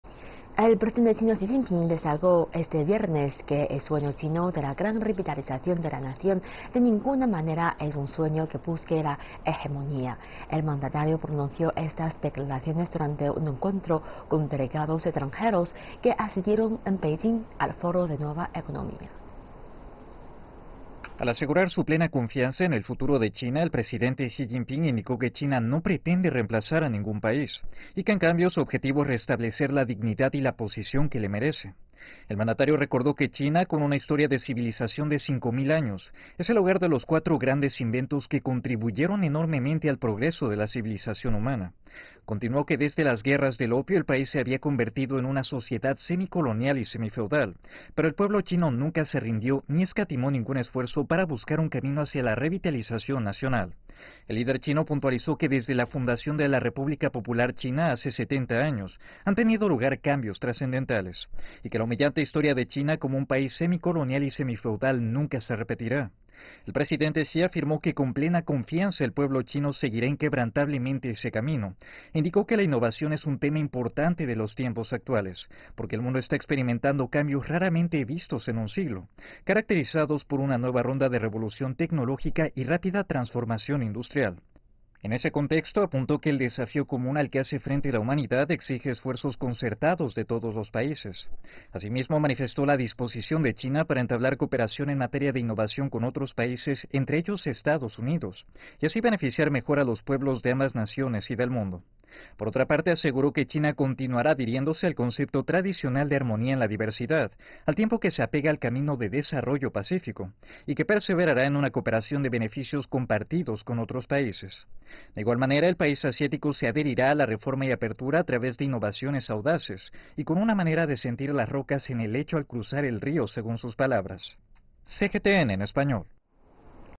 El presidente chino asegura que el sueño chino de ninguna manera es hegemónico
El presidente chino, Xi Jinping, destacó este viernes que el sueño chino de la gran revitalización de la nación, de ninguna manera es un sueño que busque la hegemonía. El mandatario pronunció estas declaraciones durante un encuentro con delegados extranjeros que asistieron en Beijing al Foro de Nueva Economía.